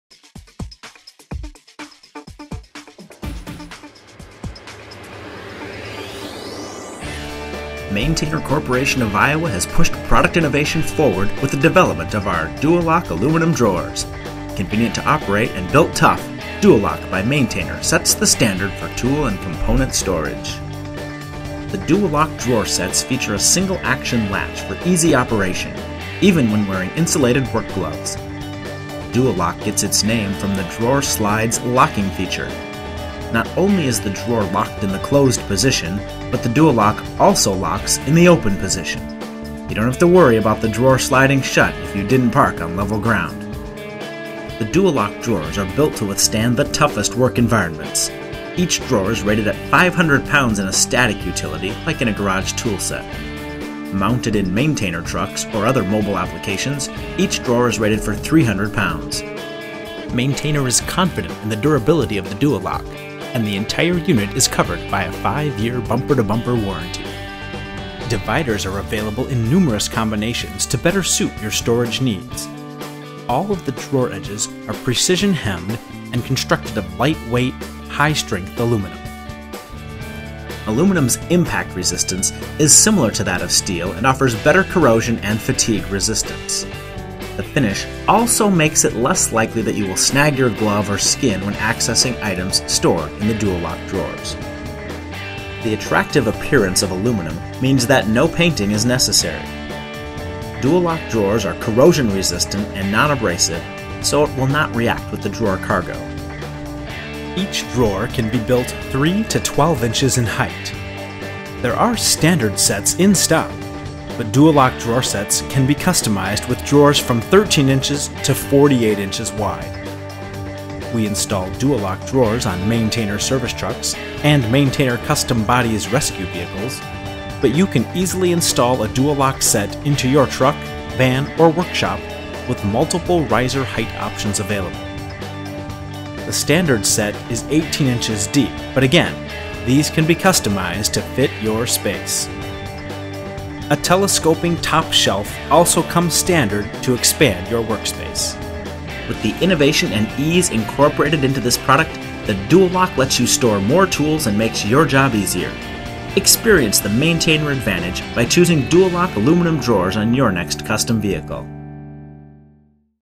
DuaLock Aluminum Drawers
INNOVATION IN ORGANIZATION - DuaLock sets the standard for tool and component storage!

SINGLE ACTION LATCH ASSEMBLY - Provides single-hand operation and locks the drawer in both the closed AND open positions, eliminating unwanted drawer closures when parked on uneven ground.

RUGGED CONSTRUCTION - Our lightweight, high-strength aluminum material saves on overall vehicle weight, but is strong enough to handle your toughest conditions. The drawers are rated for 500 lbs. in stationary applications and 300 lbs in mobile applications.

CUSTOM FIT - Standard drawer sets are available today, but we can manufacture various widths, heights, and depths to meet your exact needs.

Select a drawer set for your mechanics truck, service van, rescue truck, commercial fishing boat, or other mobile application today!

This is an UPDATE to our previous 2013 video. DuaLock now features a 5-year "bumper-to-bumper" warranty. The top shelf now comes standard. PLUS, we now have standard sets IN STOCK & READY TO SHIP!

Maintainer Corporation of Iowa, Inc.